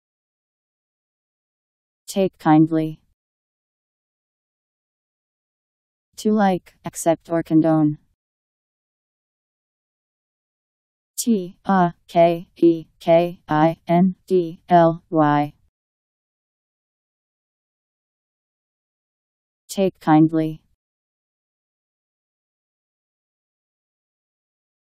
Take kindly Meaning
Video shows what take kindly means. to like, accept or condone.  Take kindly Meaning. How to pronounce, definition audio dictionary. How to say take kindly. Made with MaryTTS and Wiktionary